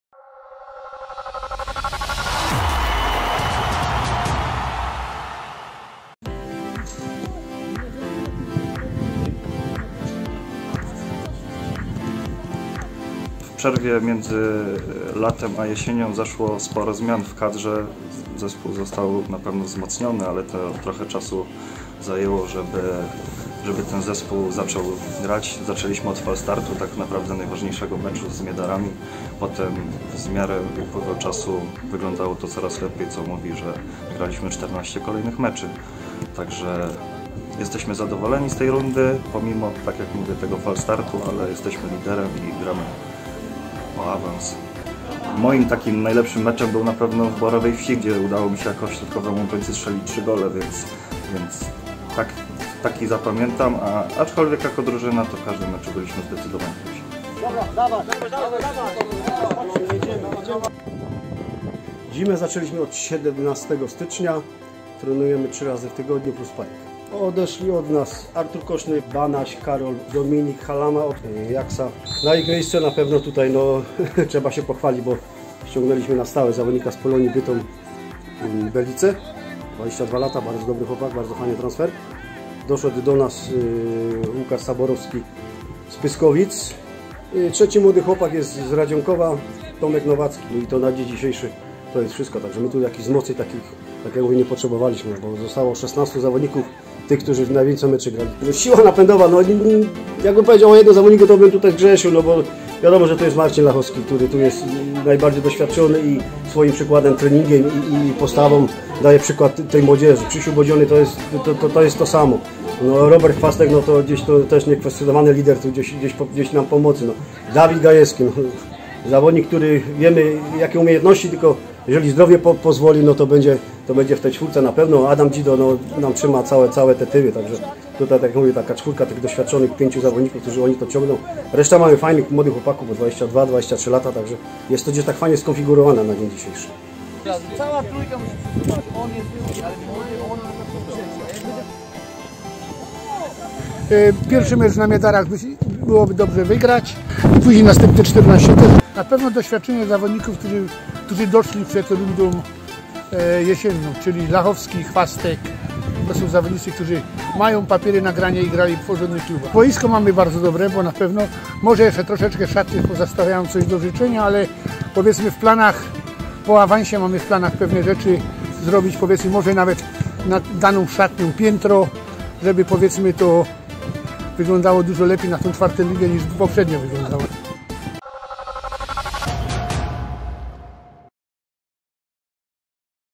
Mistrzowska przymiarka Dramy Zbrosławice
Drama Zbrosławice sięgnęła po mistrzostwo półmetka w rozgrywkach Zina Klasa Okręgowa – Grupa I Bytom-Zabrze ze znakomitą serią 14. zwycięstw z rzędu i bilansem bramkowym 67:14.

W składzie zespołu prowadzonego przez Dariusza Dwojaka są jednak tacy zawodnicy, że taki dorobek na pewno nikogo nie dziwi.
- Ta drużyna powstała latem – mówi Dariusz Dwojak. - Wtedy doszli Marcin Lachowski, Robert Chwastek, Adam Dzido czy Karol Pstuś i jakości nam przybyło, ale w okresie przygotowawczym zabrakło czasu na zgranie. Dlatego pierwszy mecz z Orłem Miedary zakończyliśmy porażką 1:2, ale później wszystko udało się już dopasować i kolejne 14 spotkań wygraliśmy wyraźnie. Takie wyniki jak 7:1 z Górnikiem Bobrowniki Śląskie czy Gwiazdą Chudów są tego najlepszym dowodem. Gdy patrzę na ten skład, który mieliśmy grając rok temu wiosną w IV lidze, z której spadliśmy, bo zabrakło nam jednego punktu i porównuję go do tego, który mieliśmy jesienią to jest ziemia, a niebo. Siłą jest doświadczenie tych, o których już wspomniałem, a także Krzyśka Bodzionego i Dawida Gajewskiego, którzy byli już u nas wcześniej. Ich połączenie z młodością, bo 22-latków też nam nie brakuje, dało efekt.